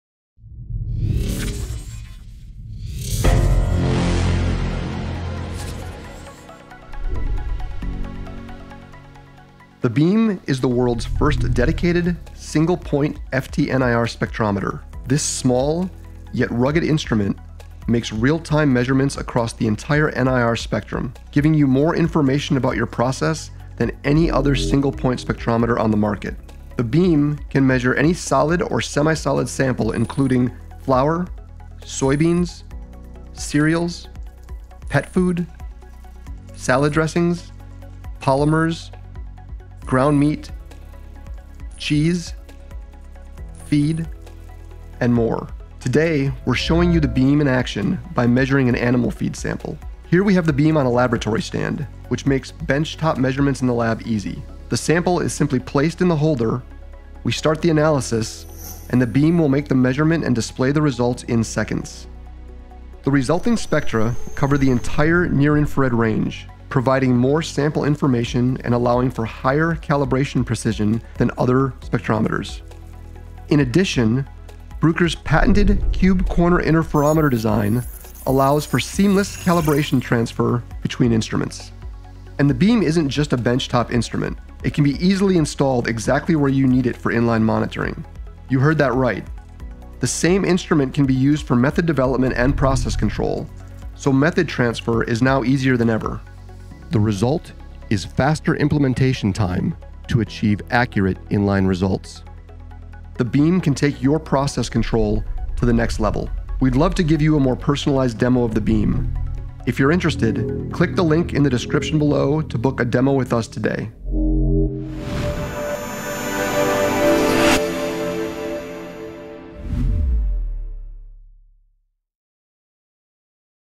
BEAM FT-NIR Process Analyzer - Demonstration
The BEAM is the world's first dedicated single-point process FT-NIR spectrometer! The BEAM measures real-time data across the entire NIR spectrum, giving you more insights into your processes than ever before. Measure a wide variety of samples from flour to polymers  to pet food in-line!
In this video, we're showcasing the BEAM in action with an animal feed sample – see how easy it is to get accurate results in just seconds! ⏱️